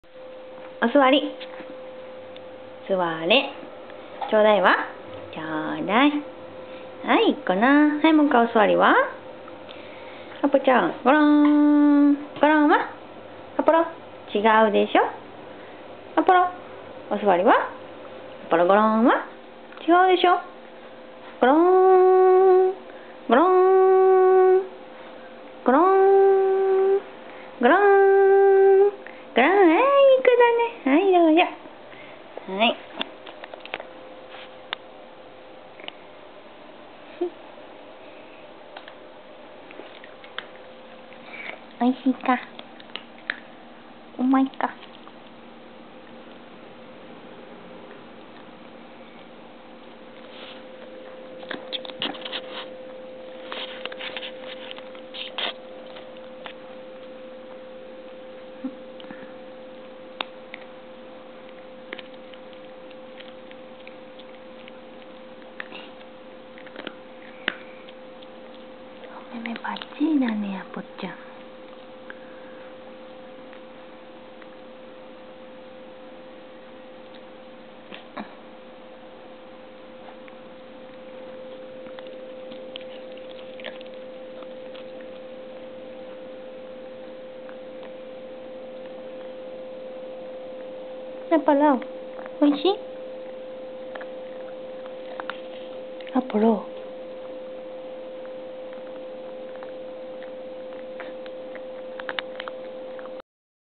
おやつ大好き♪
おやつを食べたくて芸をするんだけど。。。
テンパってなかなか上手に出来ないアポロさん。